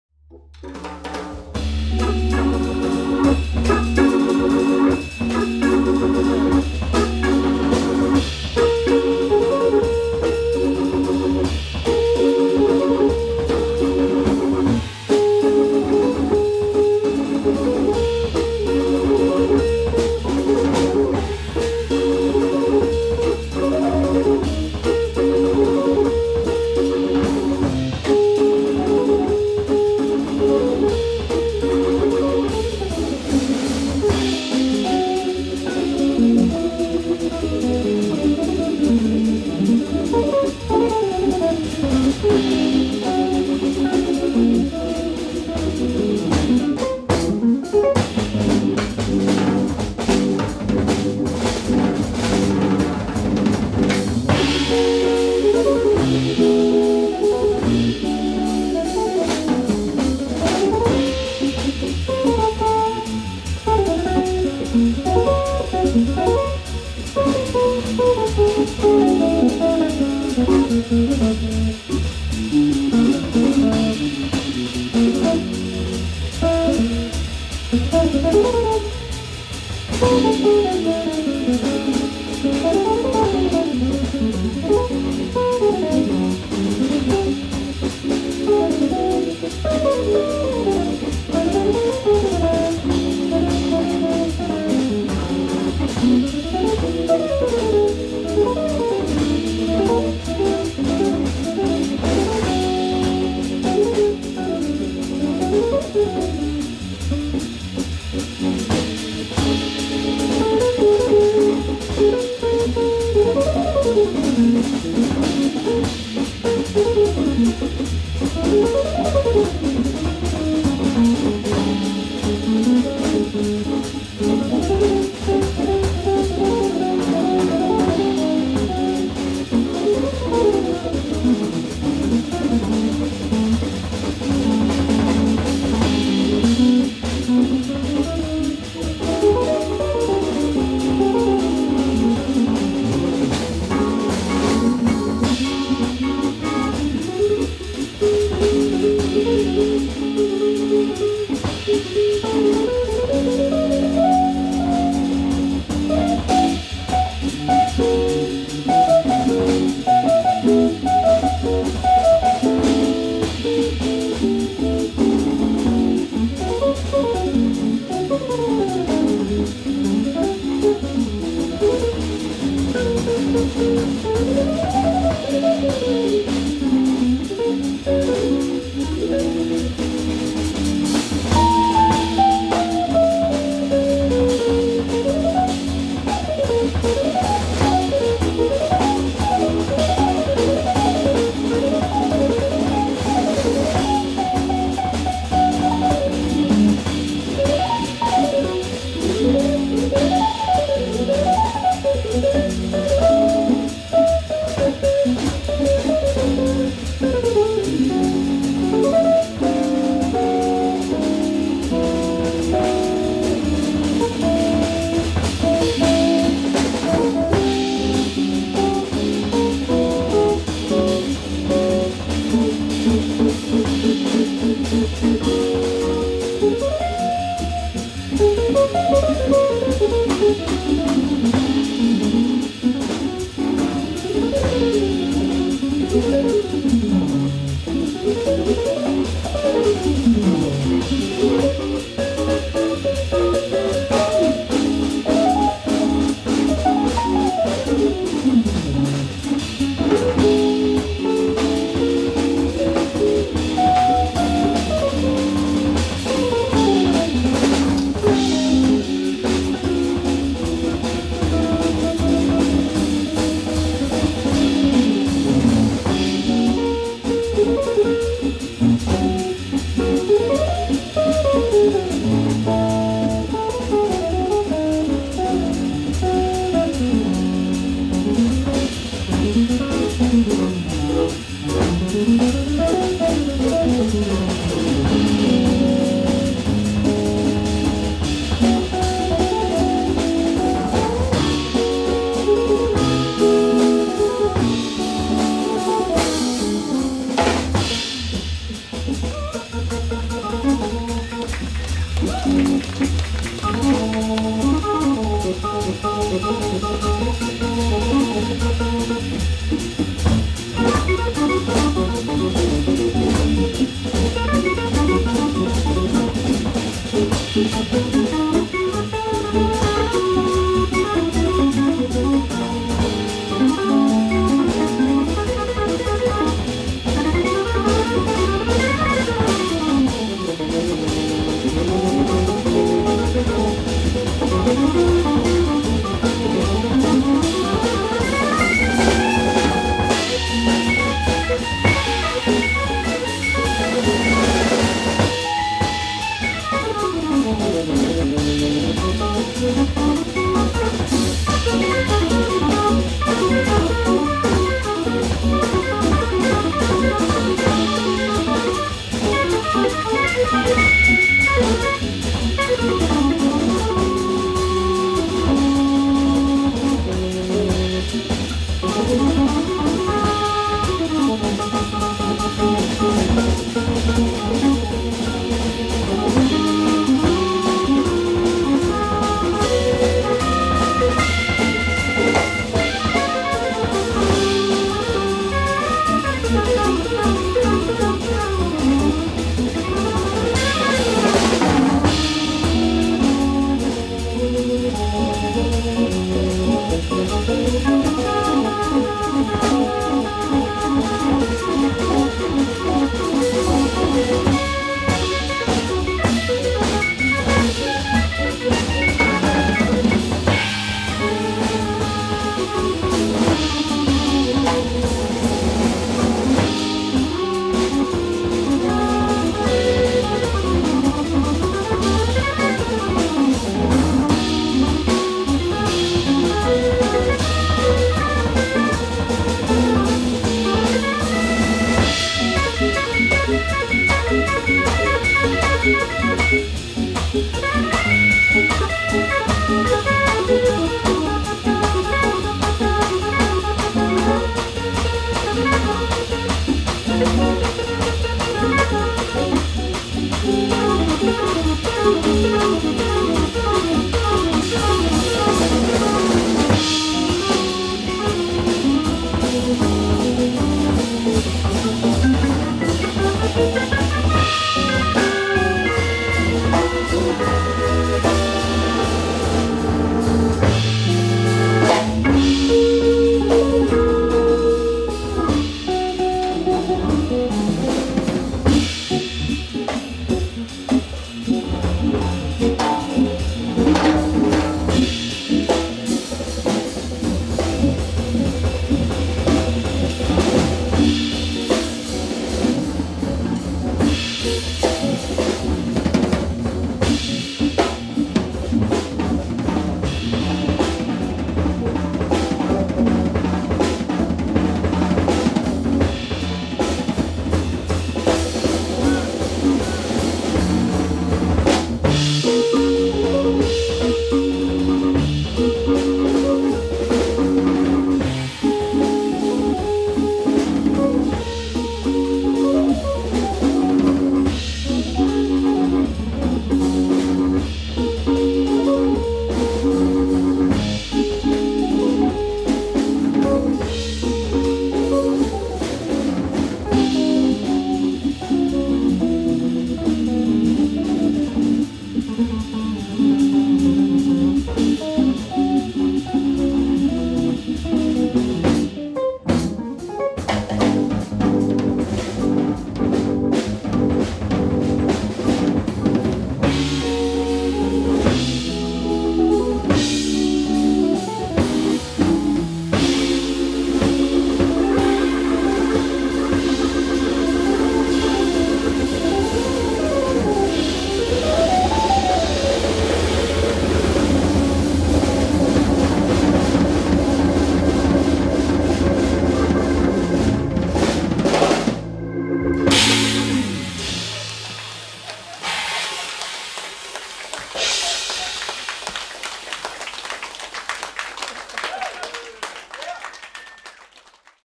ROAR BERNTSEN ORGAN TRIO | Swingn Sweet Jazzclub
LilleOleBull: 5.nov.2021 - ROAR BERNTSEN guitar -
SIMON JONSSON Hamondorgel - MAGNUS EIDE drums